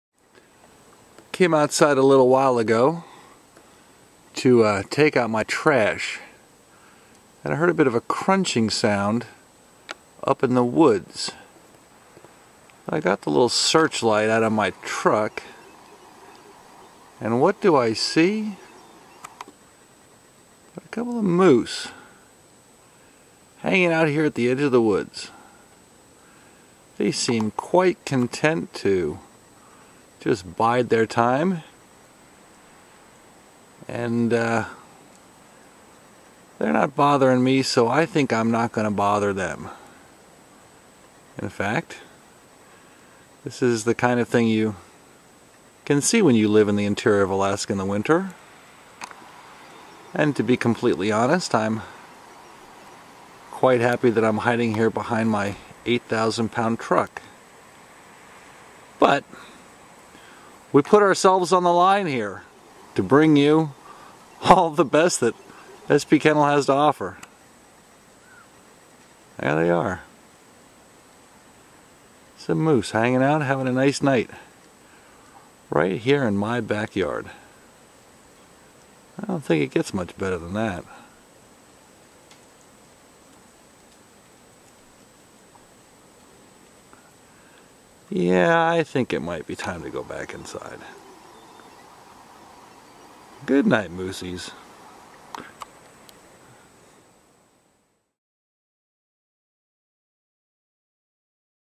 Moose In My Back Yard
-- A glimpse of winter living in the Alaskan Interior with the sled dogs of SP Kennel, Two Rivers, Alaska with Aliy Zirkle and Allen Moore